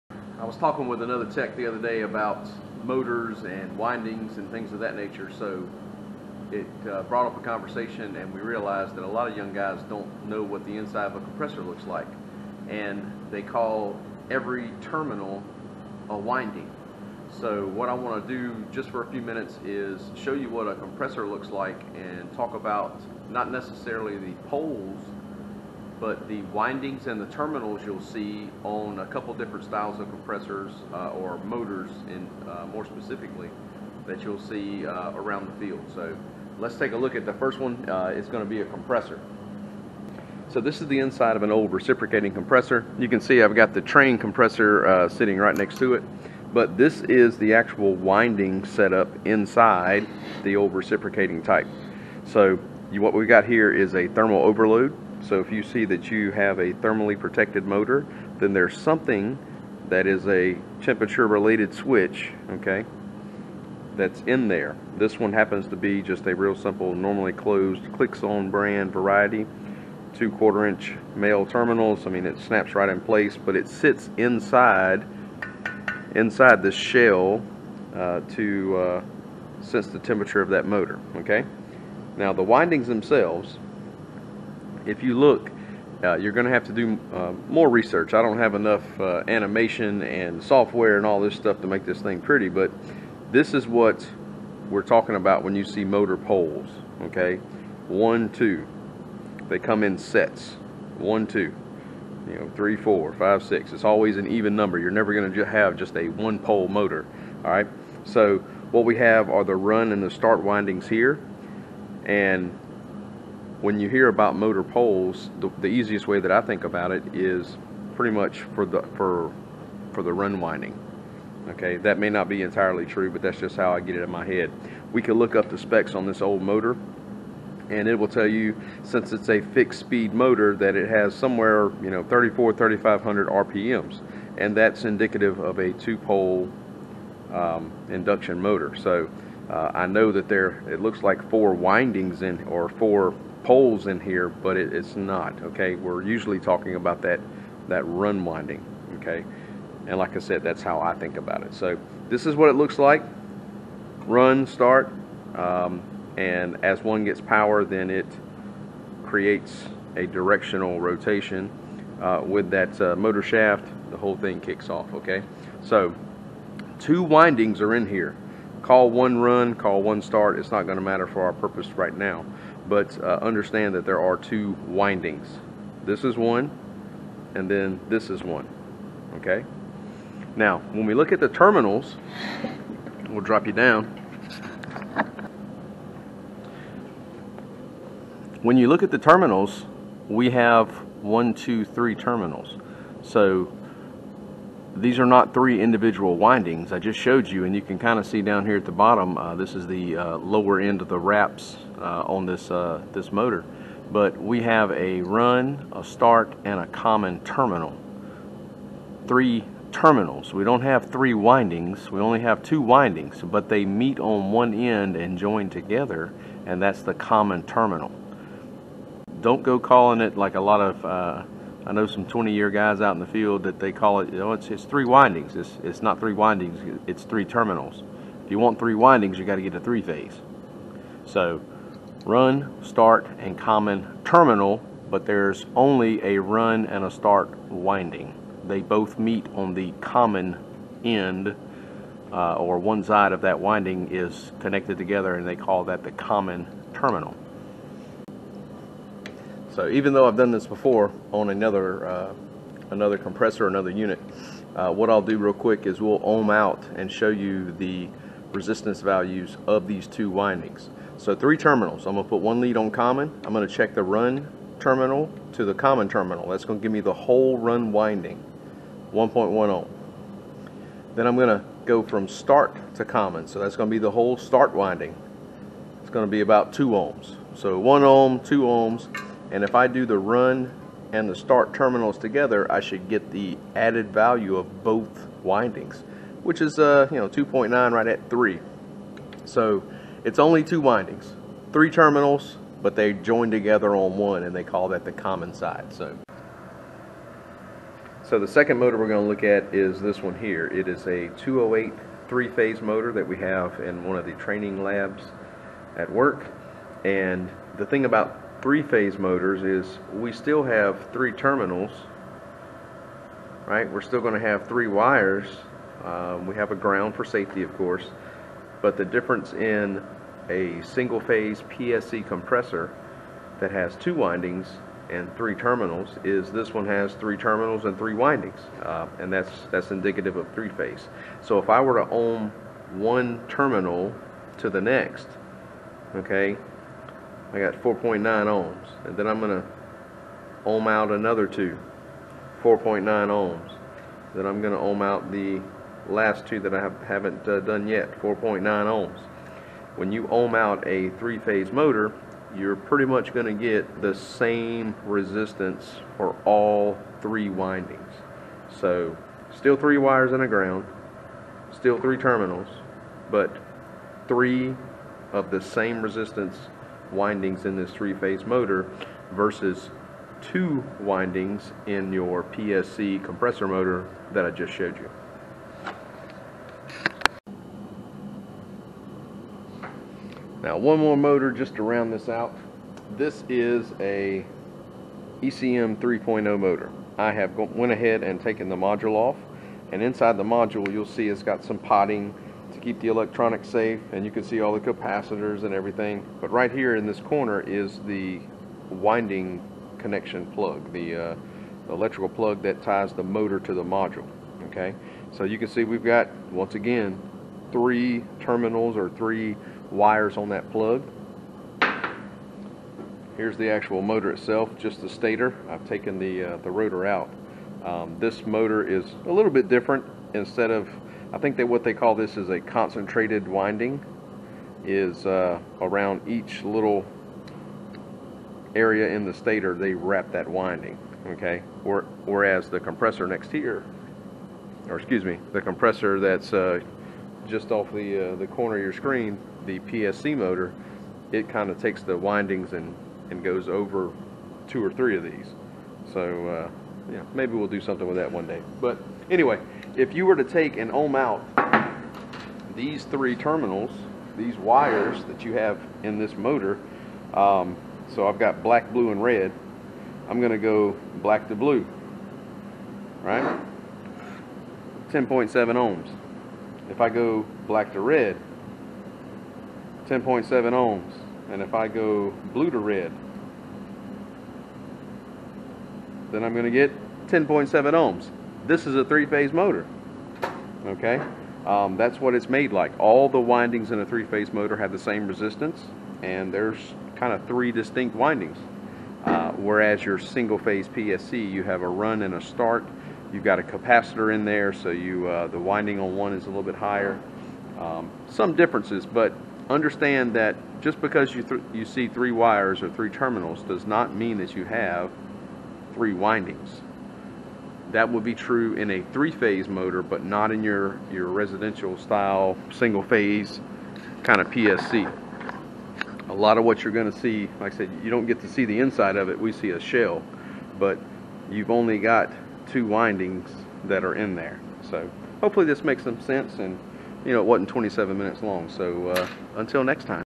Motor Windings vs. Terminals
I have been asked/talking about different motors, the amount of windings they have inside, terminals, etc. I thought it might be beneficial to put together a short video with a few examples. There are many different types of motors out there but you should try to understand the basics of them when you come across one in the field.
I started off first with the standard PSC motor, then show a 3 phase motor, and lastly a ECM motor. As with anything, this is just my 2 cent worth.
There may be a couple spots where I got close to the camera mic and the volume picks up so don't yell at me about it.

00:00 Intro
00:40 PSC Compressor
4:08 PSC Winding Check
4:56 3 Phase Induction Motor
6:17 ECM 3.0 Motor
8:22 Not a 20 min video!